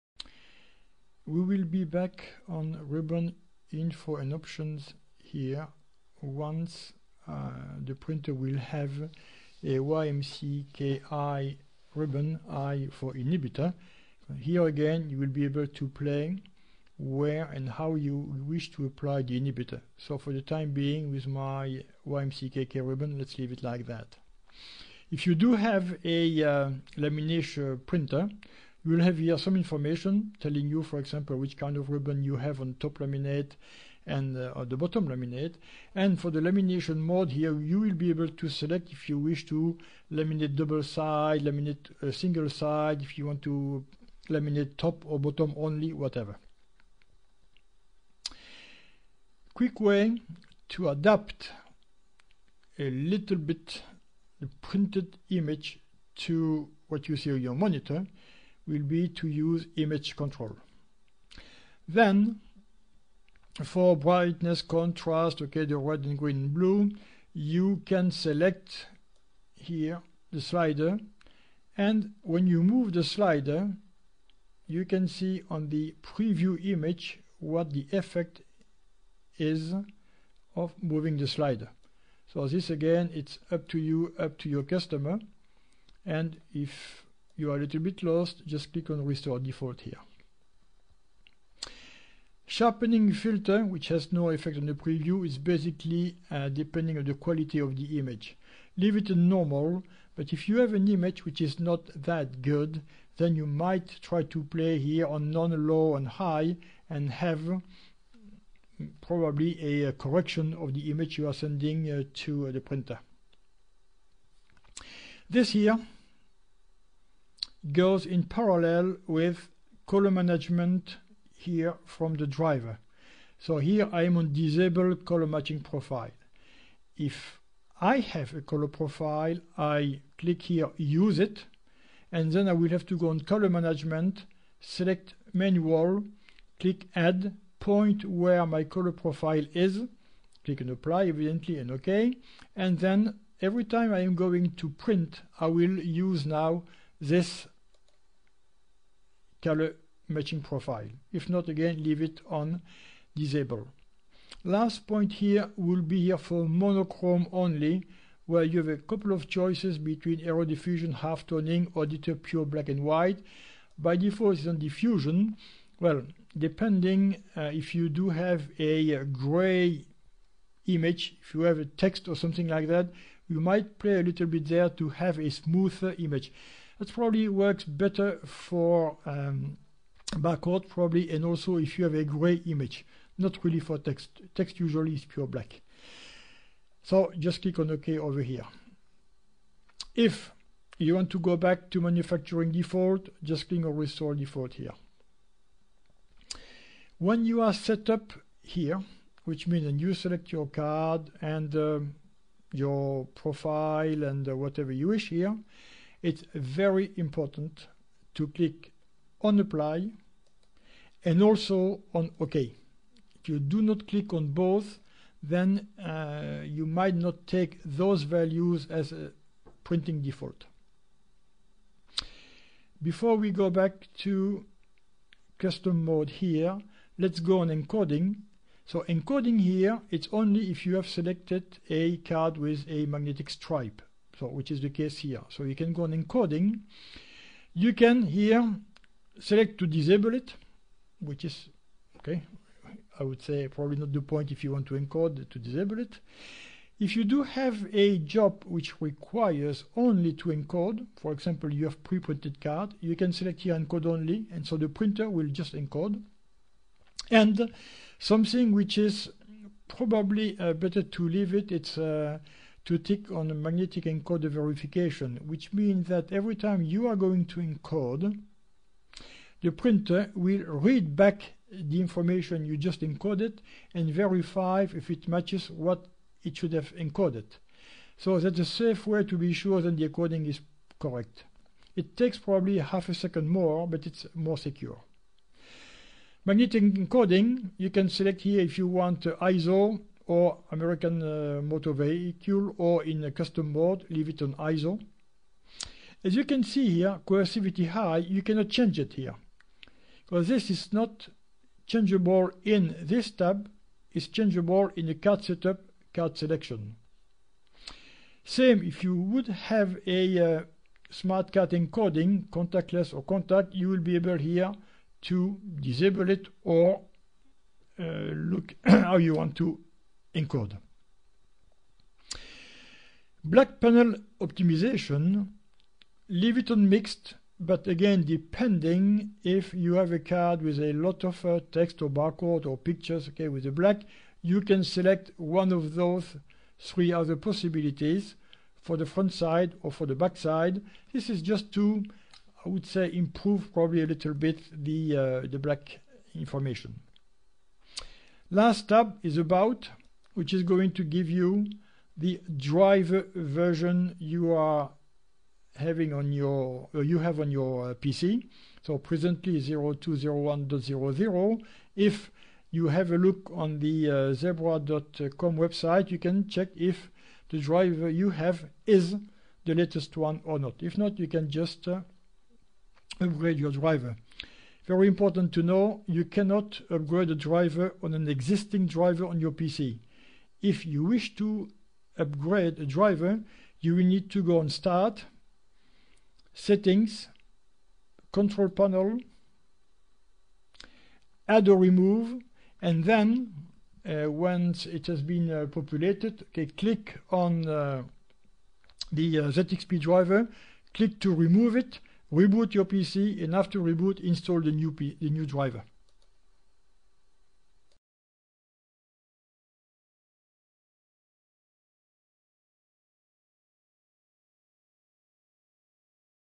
Zebra - ZXP Series 8 getting started - General Tab 3
Get started with your Zebra ZXP Series 8 printer - explore the important settings in the general tab.